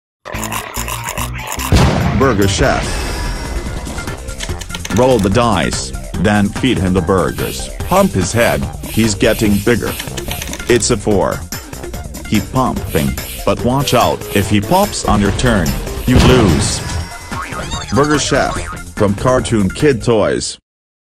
Burger Chef Ad (1999 UK)
This one had been made to make up for the 2009 Burger Chef which had been set as made for kids. (Note: I will make the remake of the 2009 Burger Chef as soon as I can.)